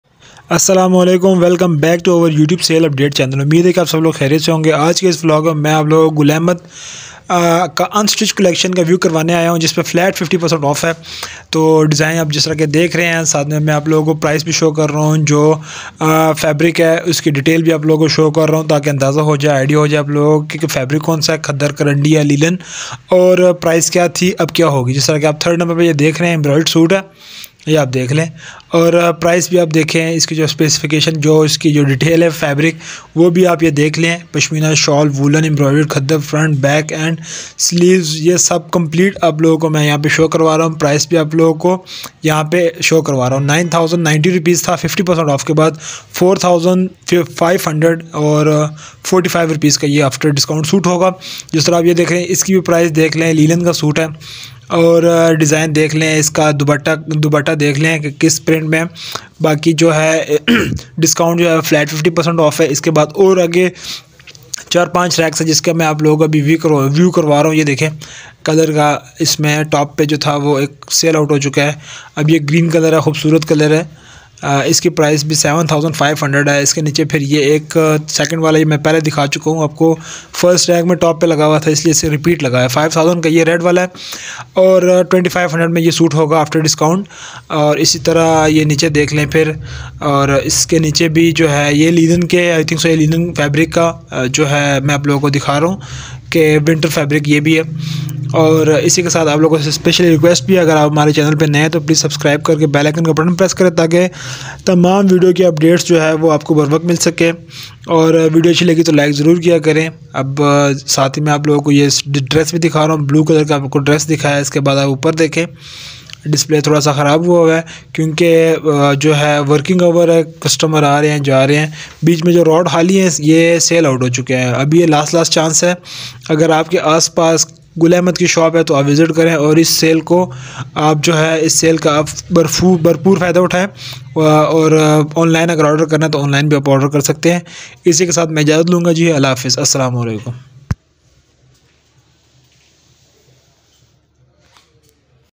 GUL AHMED sale flat 50% off Last chance || Sale Updates.
Umeed ha k ap sab log khariyat sy hong gy today es video main ap ko GUL AHMED UNSTITCHED COLLECTION dehkaon ga . And umeed karta hu k ap es video ko passand kary gy to LIKE kar k hamari hosla afzai kary  Shukria .